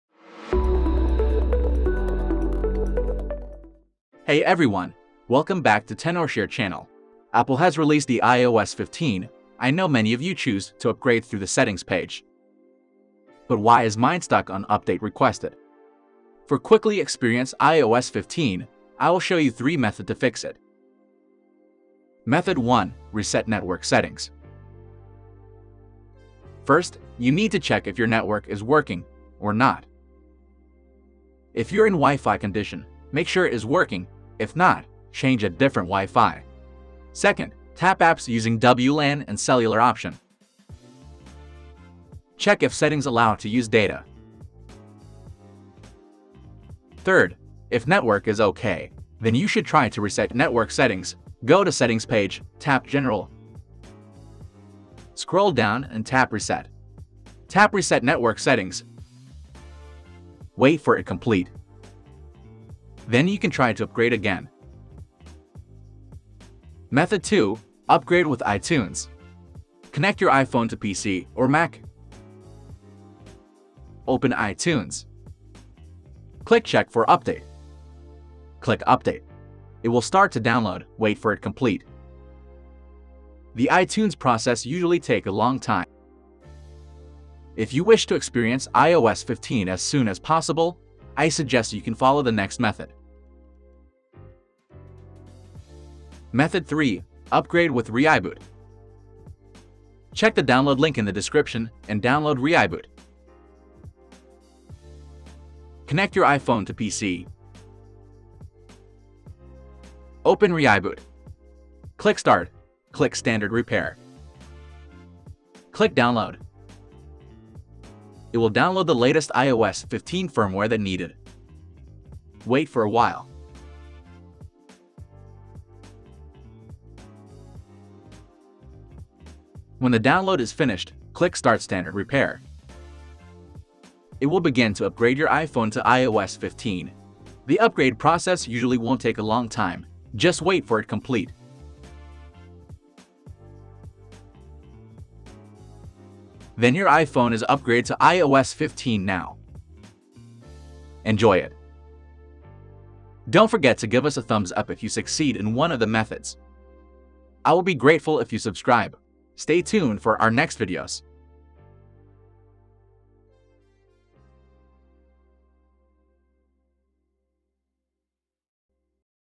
Fix iOS 15/iOS 16/iOS 17 Update Stuck on Estimating Time Remaining/Update Requested
Ready to get your hands on iOS 16 but Stuck on Updated Requested or Estimating Time Remaining?

Timestamps:
0:00 Intro & Preview
0:22 Method One: Reset Network Settings
1:06 Method Two: Upgrade with iTunes
1:36 Method Three: Upgrade with ReiBoot(Faster)